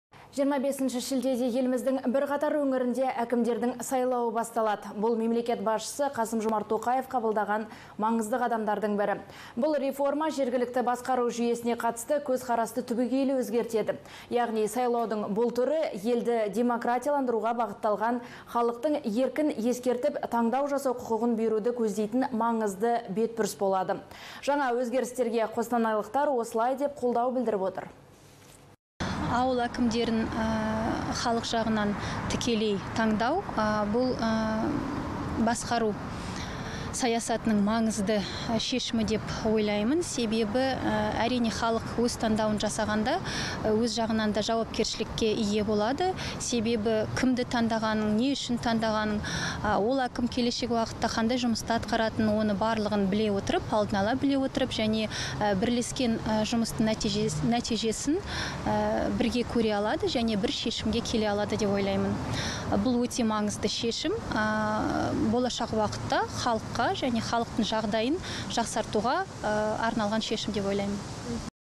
АУЫЛ ӘКІМДЕРІН САЙЛАУ: МАҢЫЗЫ БАР БЕТБҰРЫС
25 шілдеде еліміздің бірқатар өңірінде әкімдердің сайлауы басталады. Бұл – Мемлекет басшысы Қасым-Жомарт Тоқаев қабылдаған маңызды қадамдардың бірі.